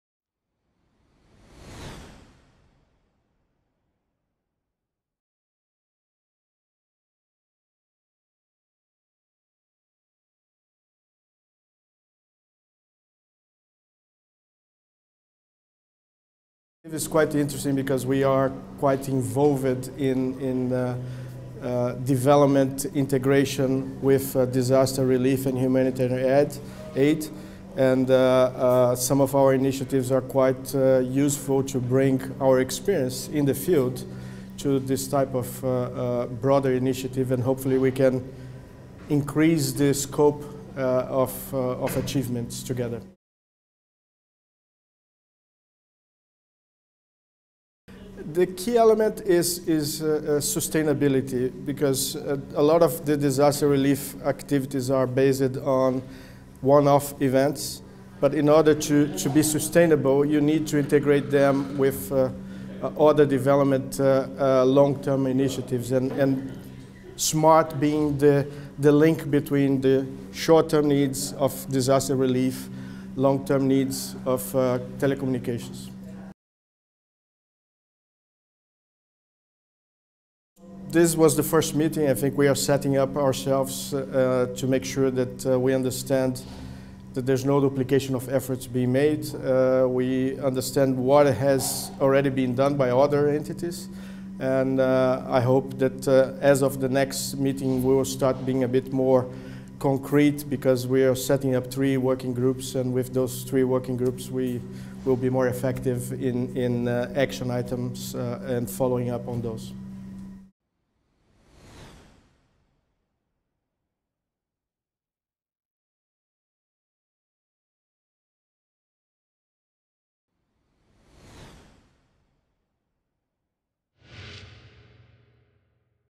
ITU INTERVIEW: Mr Gerson Souto - Smart Sustainable Development Model Initiative
Signed under a partnership agreement during the during ITU Telecom World 2012, the Smart Sustainable Development Model (SSDM) initiative seeks to demonstrate that linking ICT for Development (ICT4D) with ICT for Disaster Management (ICT4DM) leads to increased sustainable development and optimal resource use without additional financial investment. Smart solutions envisage affordable models for ICT infrastructure development, network security and enhanced cyber-security as well as the development and deployment of new applications, creation of an enabling environment through sound regulations, capacity building and mitigating the effects of climate change while strengthening emergency telecommunications.